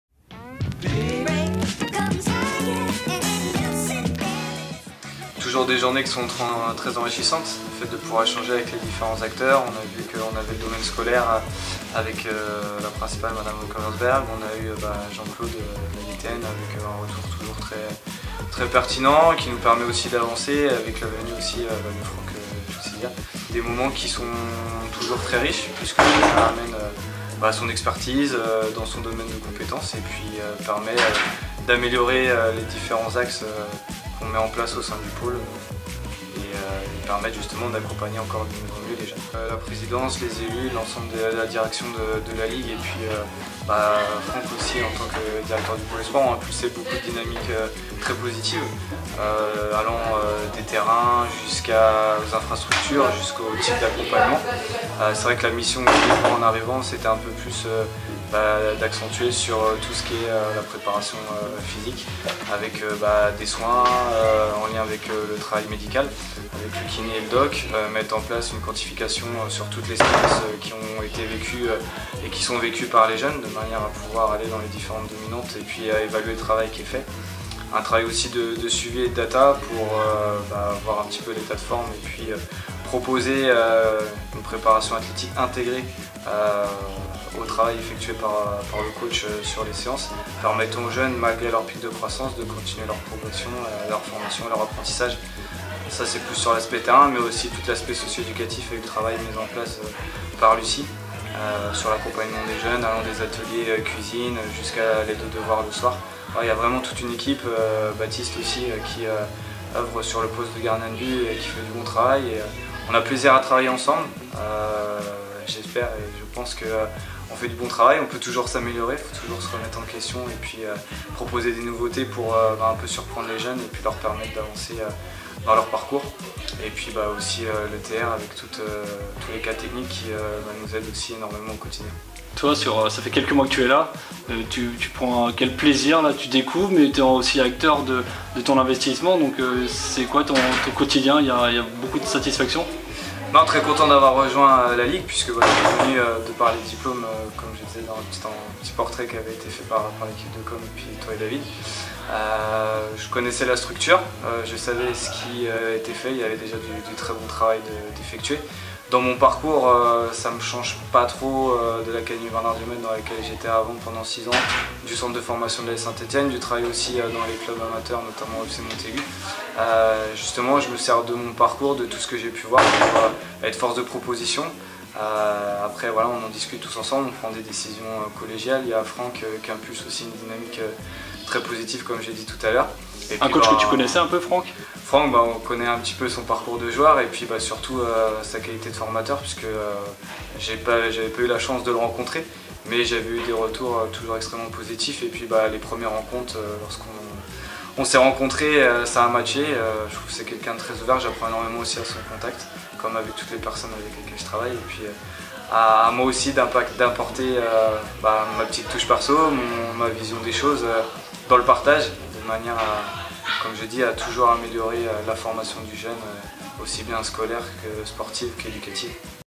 Pôle Espoirs : à la rencontre de Ludovic KÜCK (Adjoint Pôle Espoirs)
A LA RENCONTRE DE LUDOVIC KÜCK !

Très récemment Jean-Claude GIUNTINI et Franck CHAUSSIDIERE, chargés de la labellisation des Pôles Espoirs à la DTN, ont effectué leur visite annuelle au Pôle de la Ligue basée à de Saint-Sébastien sur Loire depuis sa création en 2008.

L'occasion de revenir sur cette journée avec Ludovic Kück, nouvel adjoint de Franck Maufay (Directeur du Pôle) depuis l'été dernier et qui intervient au quotidien auprès de nos jeunes internes.